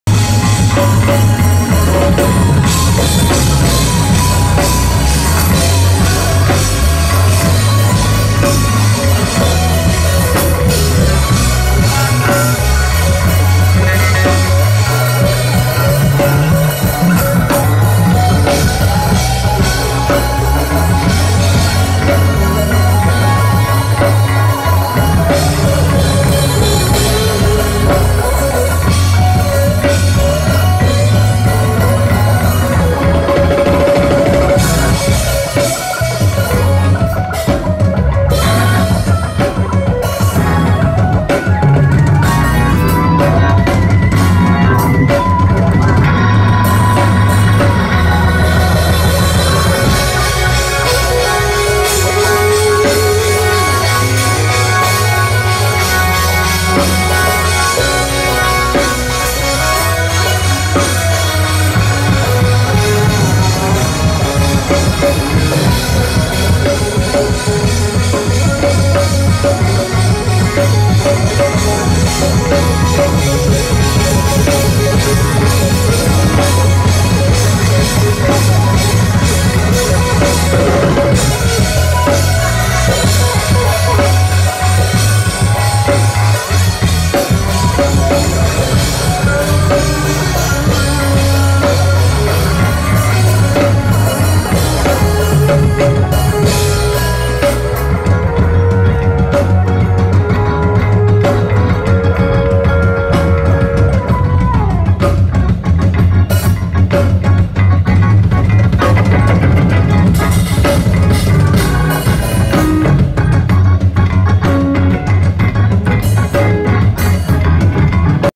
TBC Band on New Years Eve featuring Mike Neely/ Guitar
Ben Frye - Music Director/Organist
Mike Nealy - guitar
Larry Floyd- bass
Sheldon “Scoot” Costner- drums